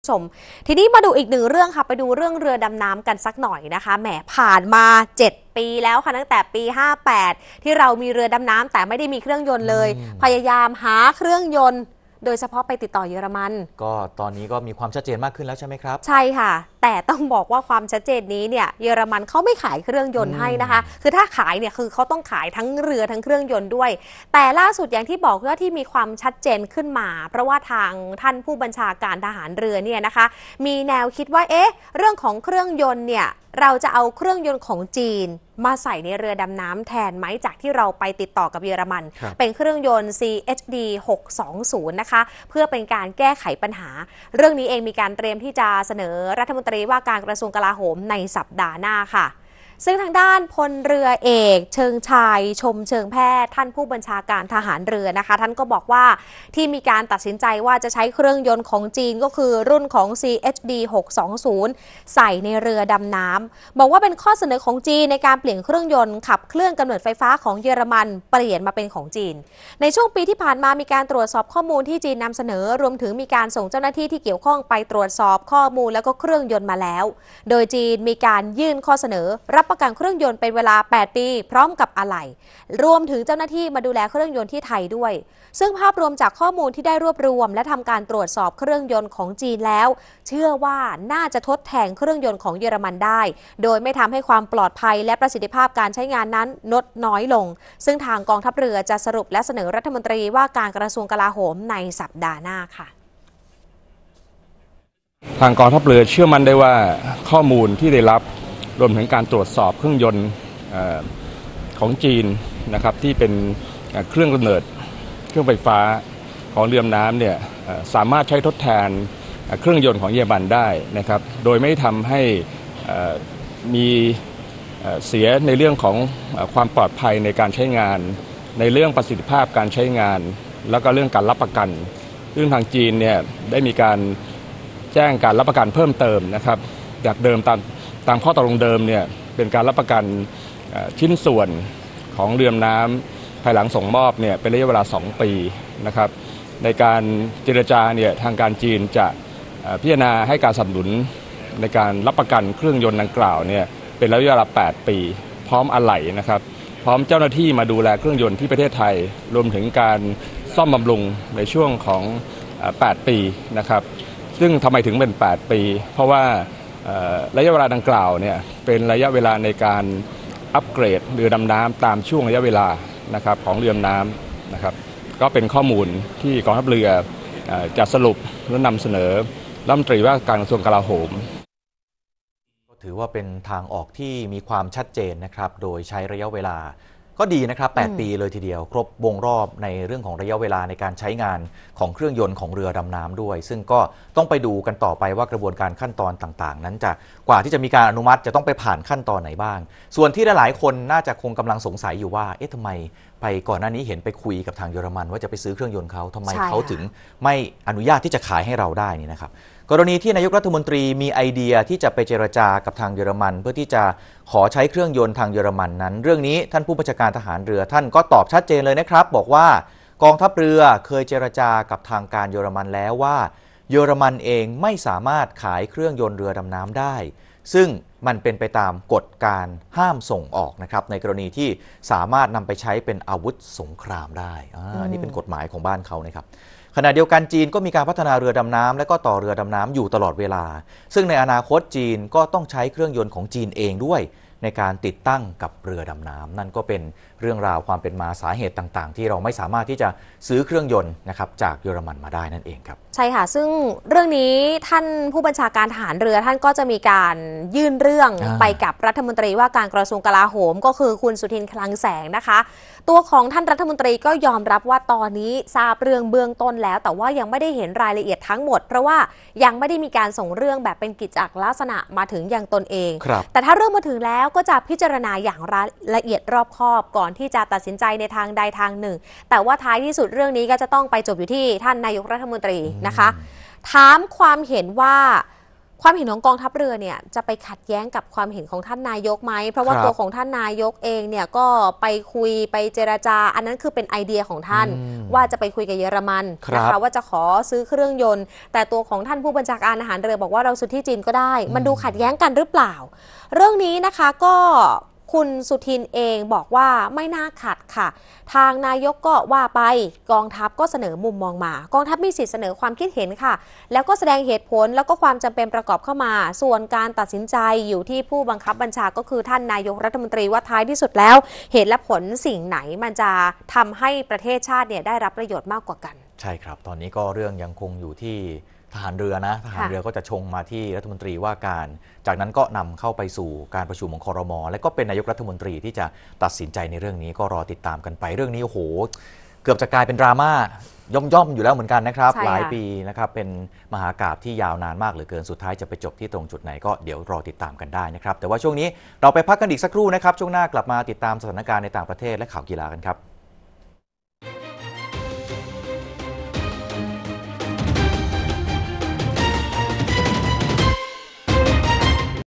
ทร.เตรียมเสนอ ครม.ใช้เครื่องยนต์จีนในเรือดำน้ำ ข่าวดึก วันที่ 21 กันยายน 2566
ปัญหาเรื่องเรือดำนำที่ยังไม่มีเครื่องยนต์ เนื่องจากทางการเยอรมัน ไม่ยอมขายเครื่องให้นั้น ล่าสุด วันนี้เริ่มพอมีความชัดเจนในระดับหนึ่งแล้วว่า ทางผู้บัญชาการทหารเรือ มีแนวคิดที่จะใช้เครื่องยนต์ CHD 620 ของจีนมาใส่ในเรือดำน้ำแทน เพื่อแก้ปัญหา โดยเตรียมที่จะเสนอรัฐมนตรีว่าการกระทรวงกลาโหมในสัปดาห์หน้า